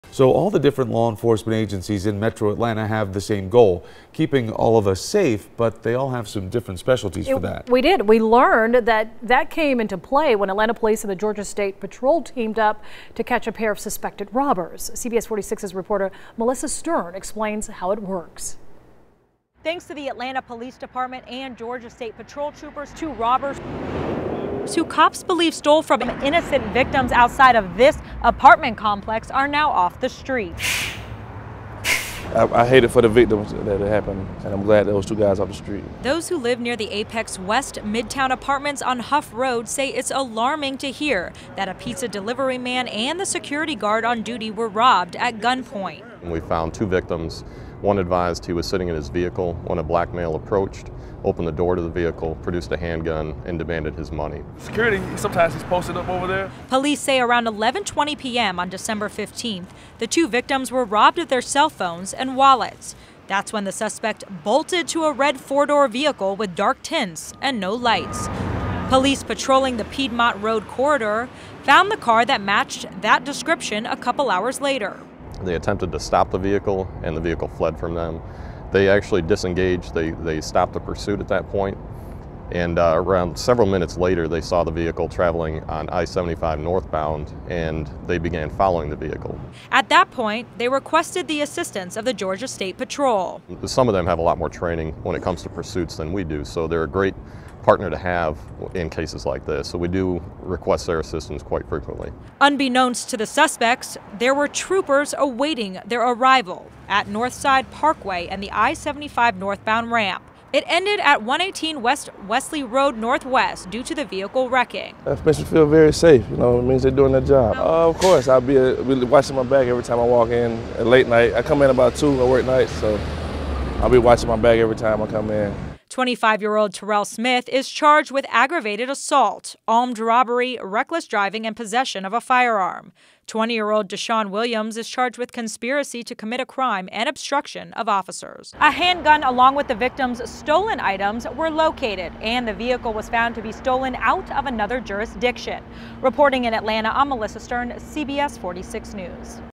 Police nab suspects accused in rash of robberies at Midtown apartment complex
Midtown